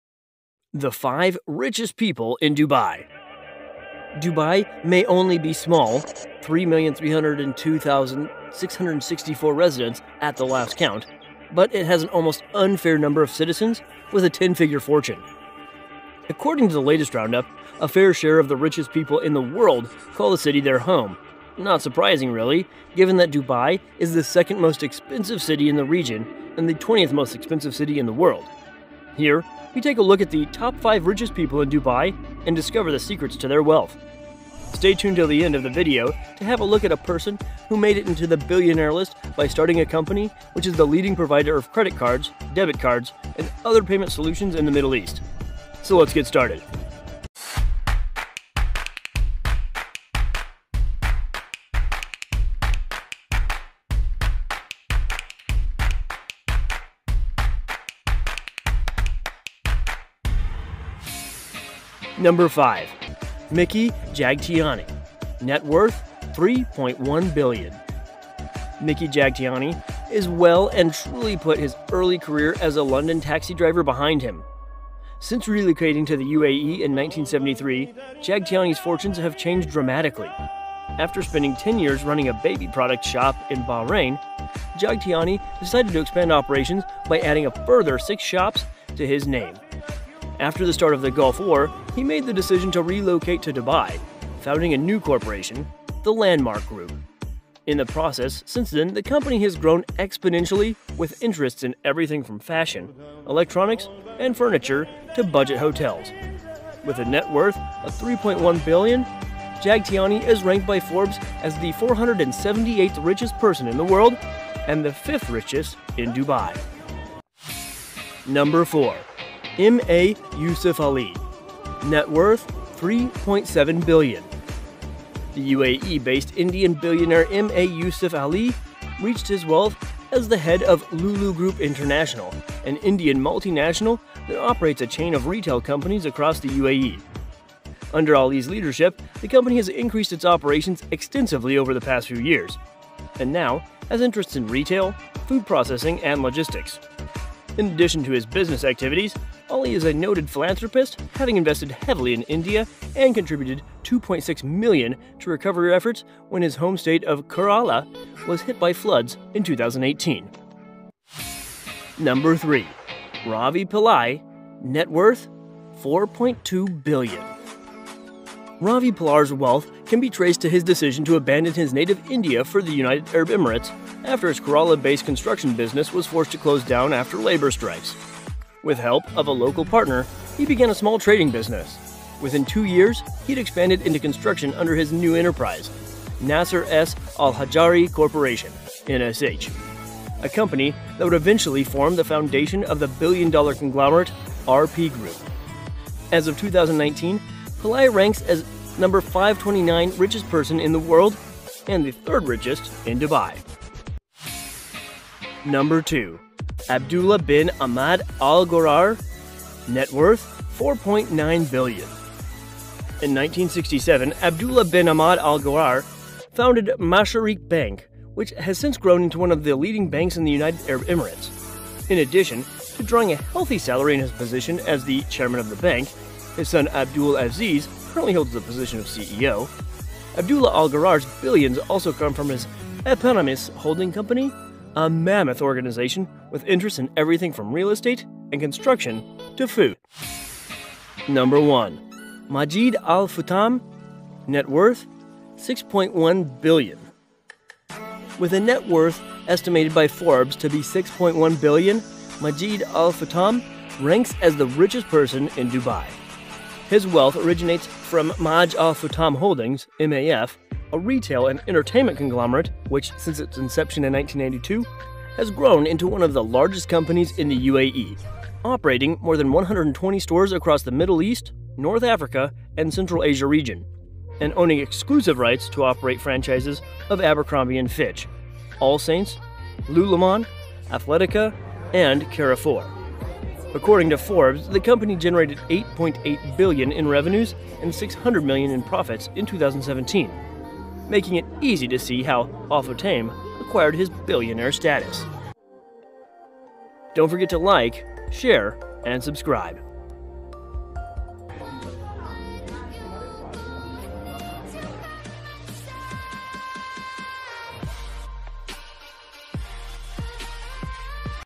Wealthiest People in Dubai | Gold Plated Sand
Dubai may only be small (3,302,664 residents at the last count), but it has an almost unfair number of citizens with a ten-figure fortune. According to the latest roundup, a fair share of the richest people in the world call the city their home- not surprising really, given that Dubai is the second most expensive city in the region and the 20th most expensive in the world. Here, we take a look at the top 5 richest people in Dubai and discover the secrets to their wealth. Stay tuned till the end of the video to have a look at a person who made into the billionaire list by starting a company which is the leading provider of credit cards, debit cards and other payment solutions in the middle-East.